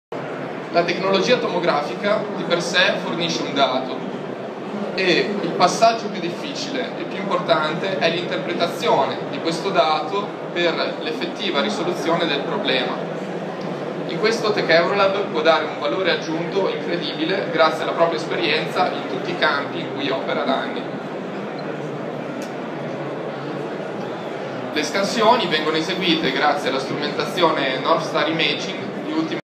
TEC-Eurolab: Tomografia Industriale - servizi integrati (MadeExpo 2013)
Nell'ambito del MADE expo - Milano 2013, passato alla cadenza biennale per allinearsi ai più importanti appuntamenti europei del settore dell'edilizia e delle costruzioni, TEC Eurolab ha partecipato con una presentazione sul servizio d'avanguardia e di recente acquisizione, la Tomografia Industriale.

L'ing. Marco Moscatti (in foto) ha tenuto la presentazione, illustrando ai presenti le enormi potenzialità della tecnologia e gli ambiti applicativi, suscitando interesse e discussione sul tema. Qui un breve estratto durante il quale viene spiegato come TEC Eurolab unisce alla Tomografia Industriale, le proprie competenze integrando alcuni dei servizi a più alto valore aggiunto per un supporto completo al cliente.